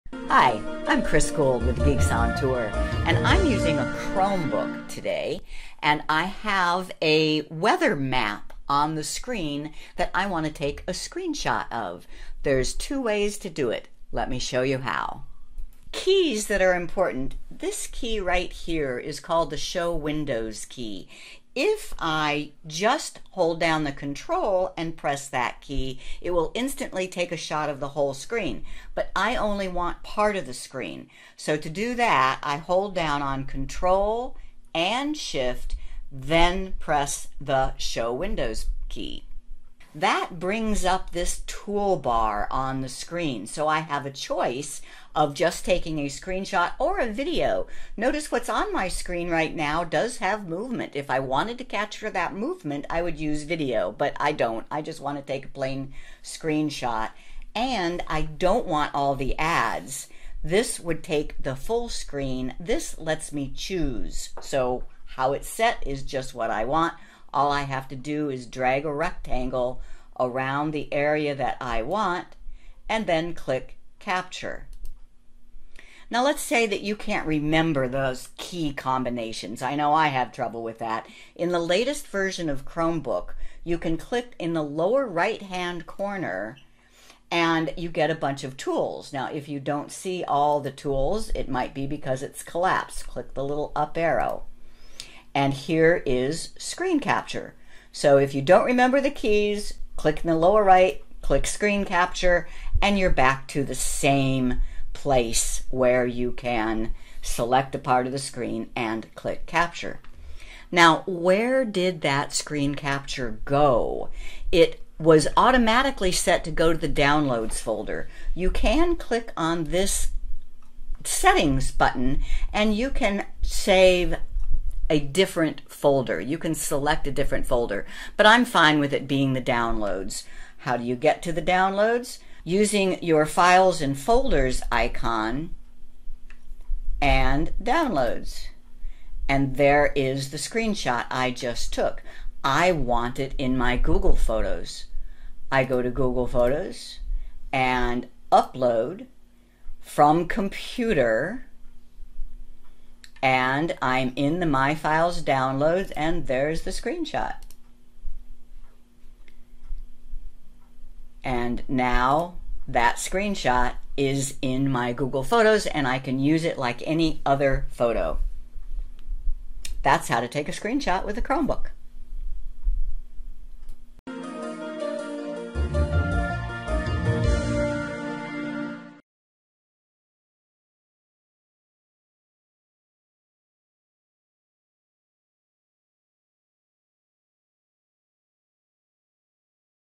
How to Take a Screenshot on a Chromebook Tutorial Video 742
You will learn how to get a screenshot of a Chromebook screen.